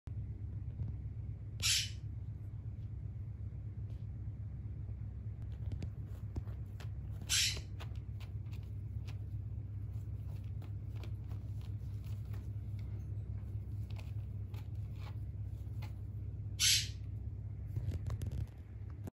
やってくれました【ヨウム飼育記録】
吾輩の寝室にうん○💩されました。
我家のボス、フータさんも勝手に入って来ないのに、うん○💩されまし😨